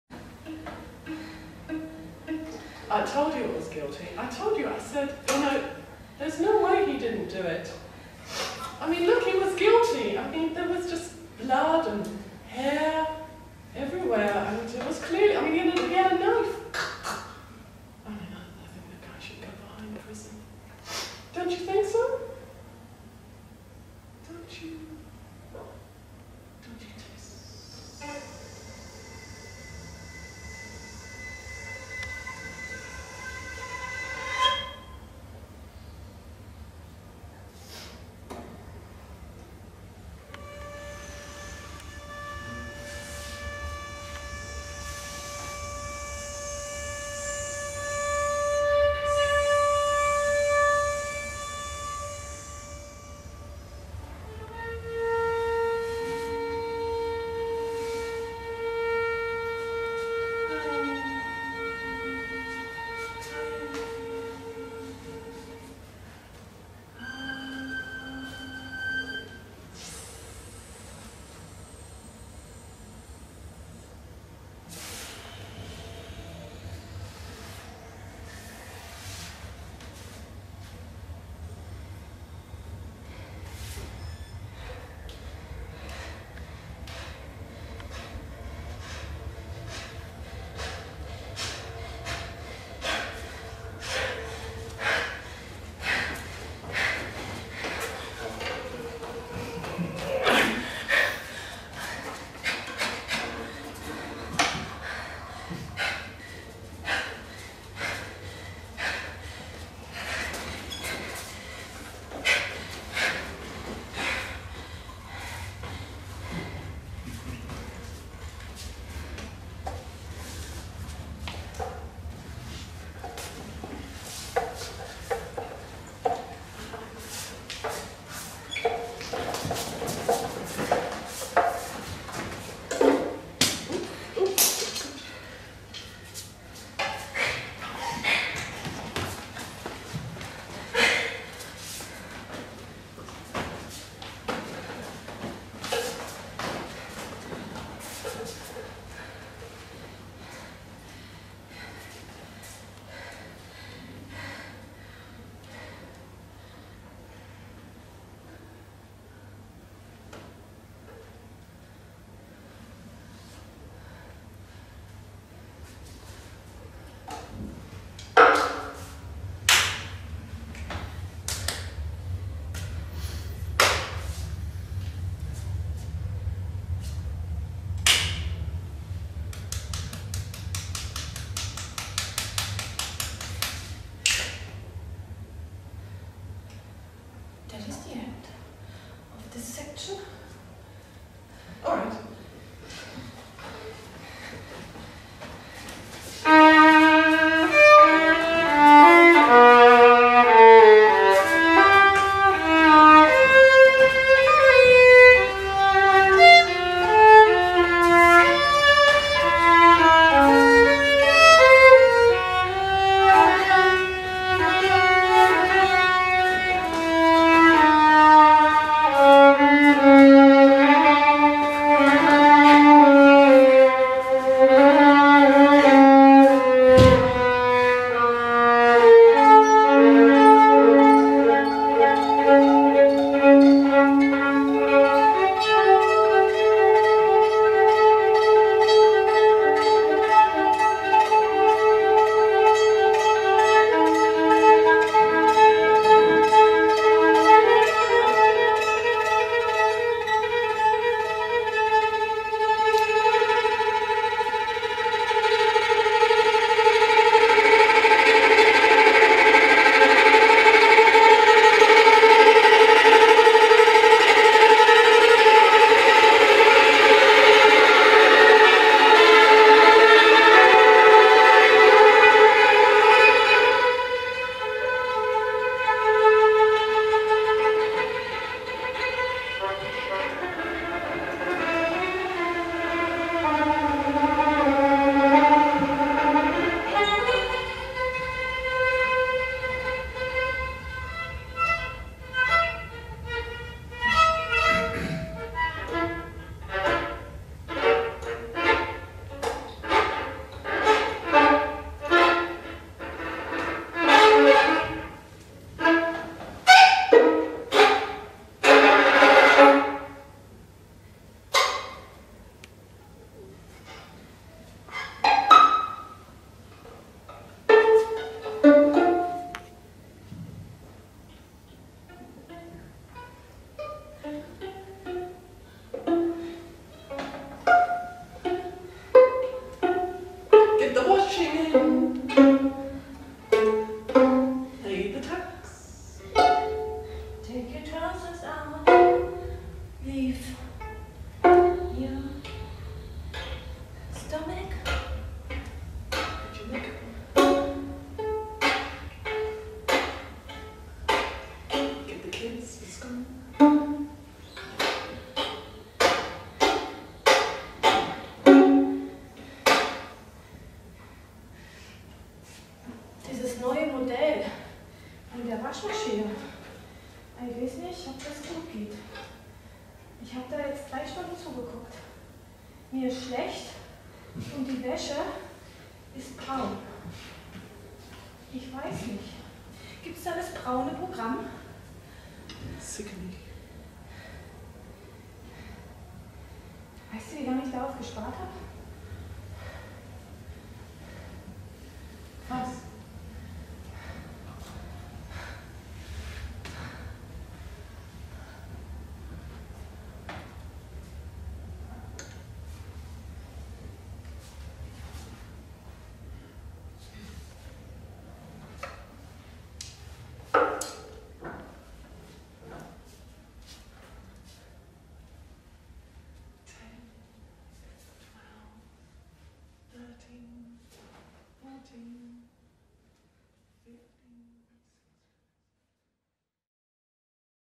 FIM - HAPPY NOW? - ein Duo über drei Medien
Kostprobe aus HAPPY NOW? mit
Uta Salewski  -- Tanz und Performance
Emma McClune -- Violine und Performance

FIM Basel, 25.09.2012